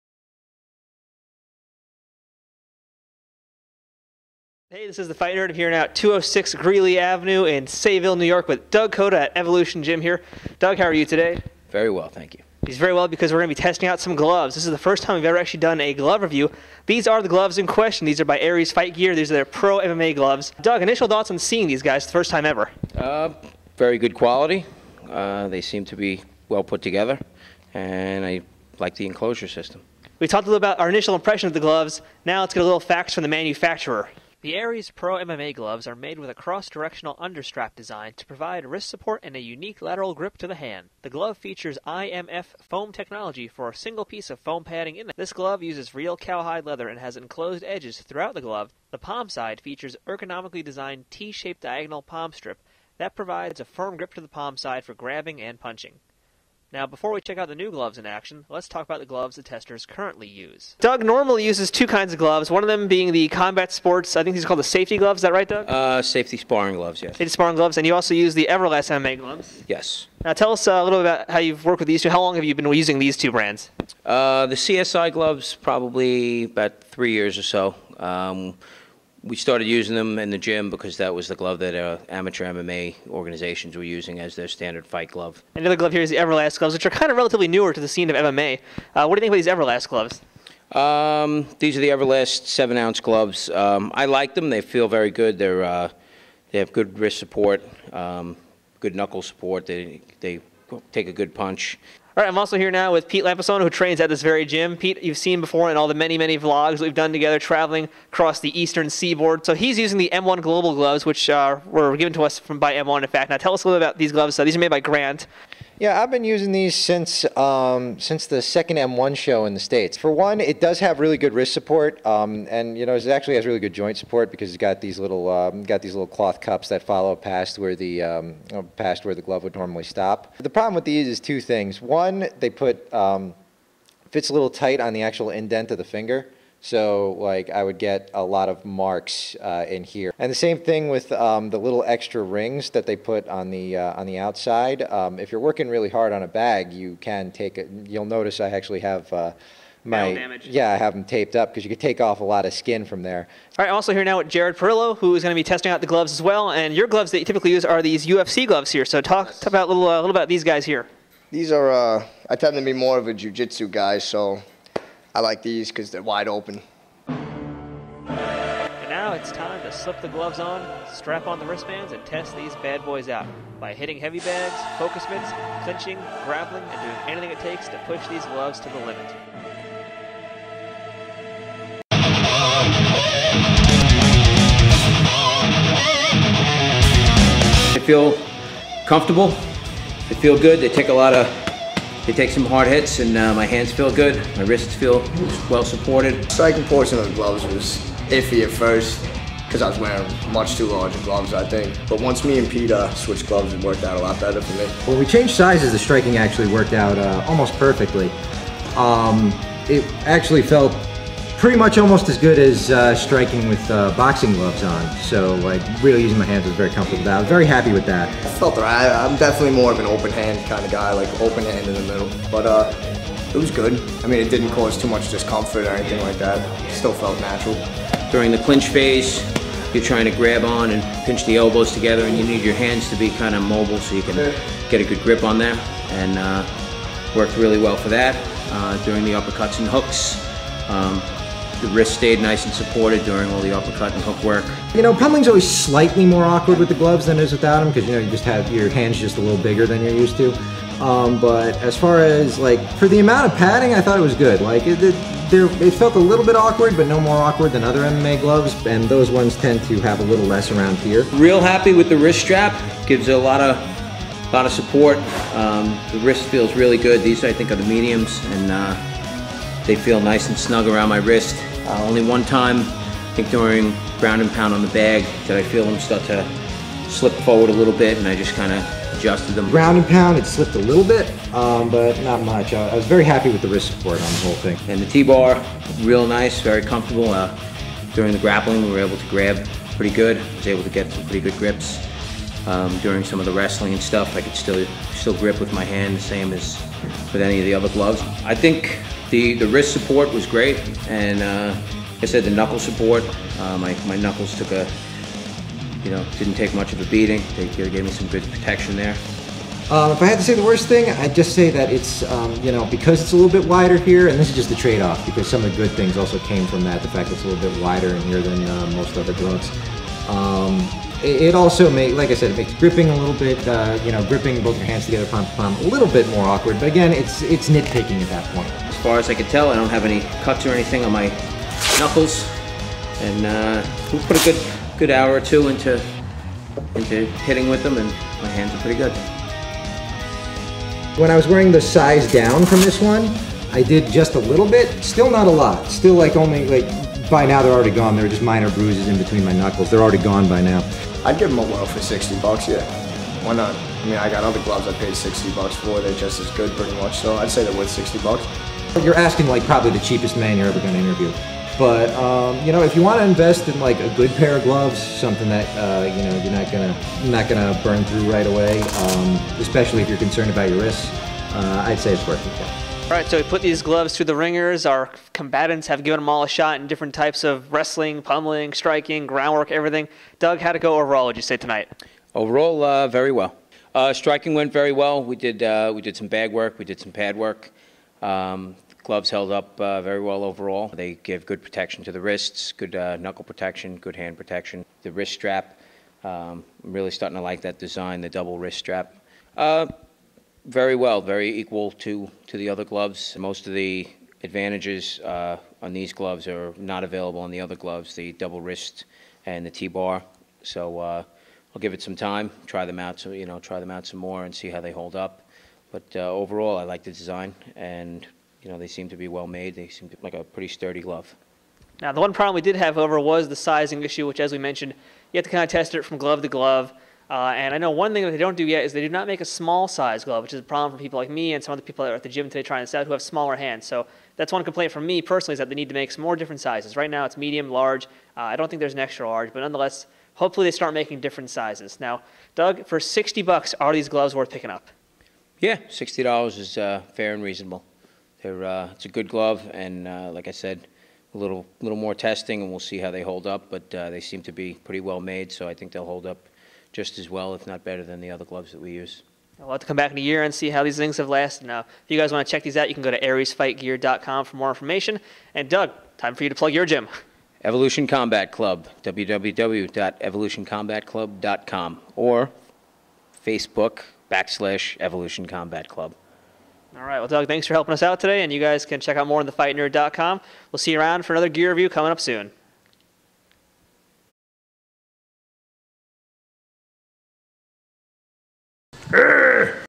Aries Fight Gear Pro MMA gloves review
The Fight Nerd returns with another gear review! This time, we take a look at a pair of professional MMA gloves from Aries Fight Gear. This unique looking glove is a pricey model, but is it worth the cost? We head over to Evolution Combat Club in Sayville, New York and put these to the test through striking focus mitts, thai pads, heavy bags, clinching & wrestling and grappling to see how these hold up to a hard workout session? Could these be your next pair of MMA gloves? Watch the review to find out!